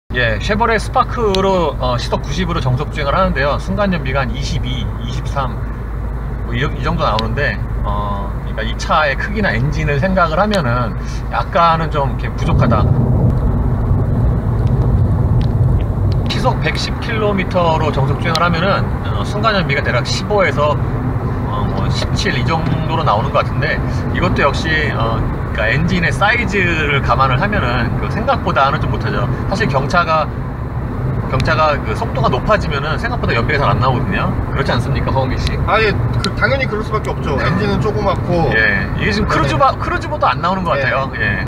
쉐보레 더 넥스트 스파크 정속 주행 연비(New Spark 1.0 Fuel Economy) - 2017.02.12
쉐보레 더 넥스트 스파크 C-테크의 정속 주행 연비입니다. 시속 90km으로 정속 주행하면 리터당 21~23km, 시속 110km에서는 리터당 15~18km 사이의 순간 연비가 찍힙니다. 보통 경차는 속도가 높으면 기대만큼 정속 주행 연비가 안 나오는 경우가 많습니다. 방금 말씀 드린 순간 연비는 얼마 전 시승했던 올 뉴 크루즈를 비롯한 준중형급보다 못합니다.
쉐보레 더 넥스트 스파크 C-테크 LTZ 주요제원
트레드 1,410/1,418mm
엔진 999cc 가솔린 3기통
최고 출력 75마력/6,500 rpm
최대 토크 9.7kg.m/4,500 rpm
변속기 CVT
공차중량 910kg
타이어 넥센 CP672 195/45R/16
연료탱크 32리터
복합 연비 리터당 14.3km(도심 13.2km, 고속도로 15.7km)
CO2 배출량 117g/km
가격 992~1,562만원